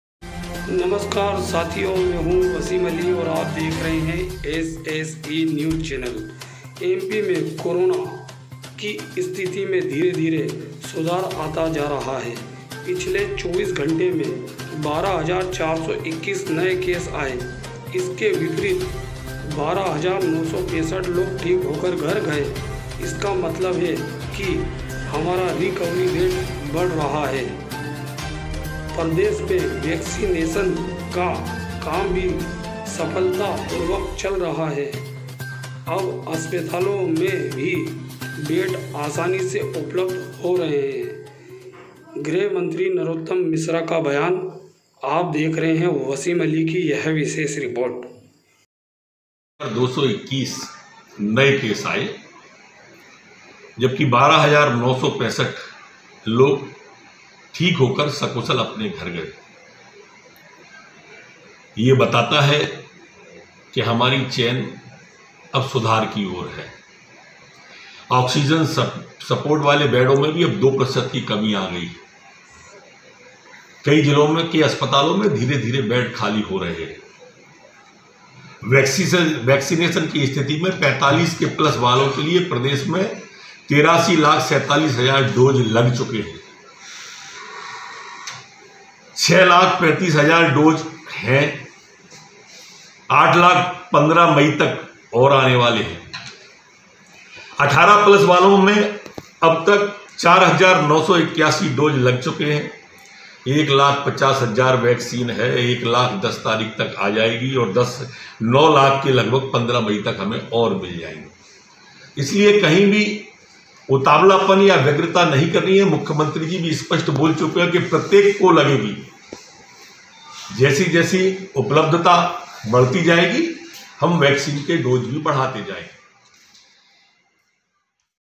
कोरोना संक्रमण की स्थिति में धीरे-धीरे आया सुधार,12965 लोग ठीक होकर गए घर : गृहमंत्री नरोत्तम मिश्रा
एमपी मे कोरोना संक्रमण की स्थिति मे धीरे धीरे सुधार आता जा रहा है...पिछले 24 घंटे मे 12421 न ए केस आए...

इसके विपरीत 12965 लोग ठीक होकर घर ग ए,इसका मतलब है कि हमारा रिकवरी रेट बढ रहा है...

आक्सीजन सपोर्ट वाले बेड मे भी दो फीसदी की कमी आई है...

अब अस्पतालों मे भी बेड आसानी से उपलब्ध हो रहे हैं...

प्रदेश मे वैक्सीनेशन का काम भी सफलता पूर्वक चल रहा है...

अब तक 83 लाख 47 हजार डोज लग चुके हैं...

45 प्लस वालों का वैक्सीनेशन पहले की तरह ही जारी है...

अभी हमारे पास 6 लाख 35 हजार डोज हैं...और 8 लाख 15 मई तक आ जाएंगे..

जहां तक 18 प्लस वैक्सीन वालों का सवाल है तो 4 हजार 981 को डोज लग चुके हैं एक लाख 50 हजार वैक्सीन हमारे पास हैं...और 10 लाख 67 हजार वैक्सीन 15 मई तक आ जाएंगी...

डॉ. नरोत्तम मिश्रा,गृह एंव जेल मंत्री